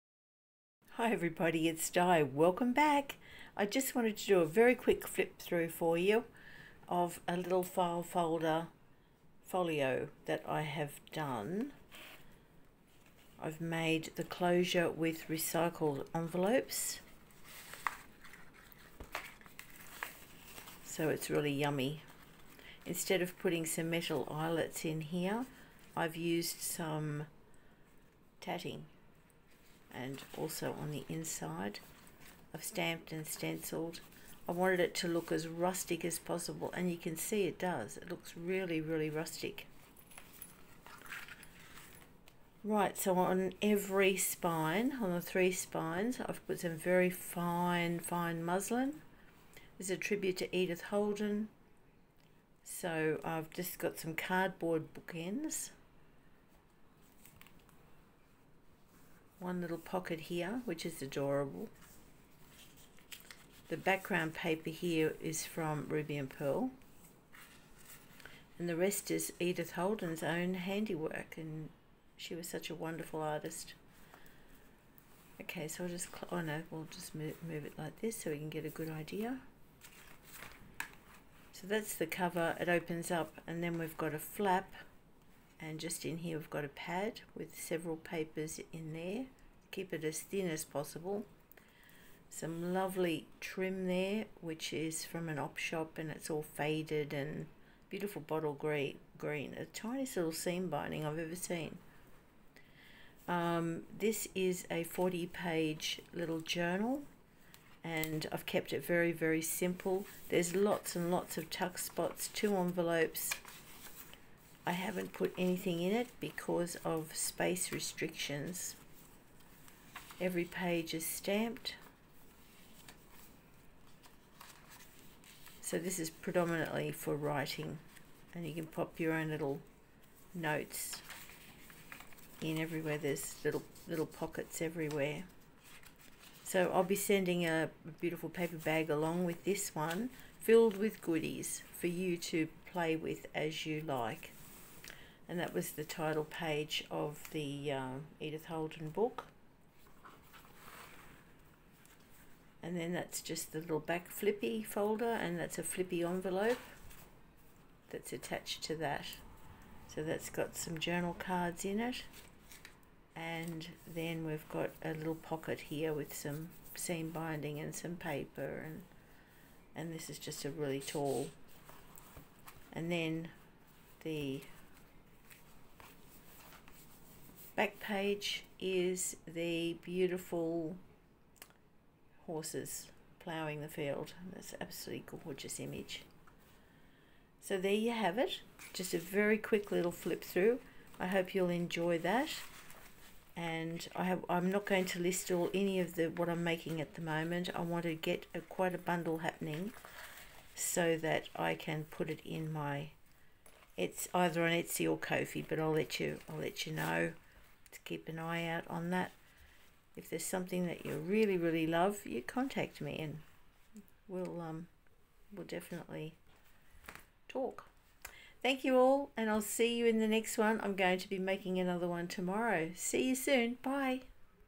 Flip through of a small folio in the vintage style.
Hi everyone, thanks so much for tuning in. This is just a very short flip through of a vintage inspired folio made from a file folder. It is 8" x 4" and has a 40 page plain journal with plenty of tuckspots, but predominantly writing space. I do hope you like it. Cheers, Di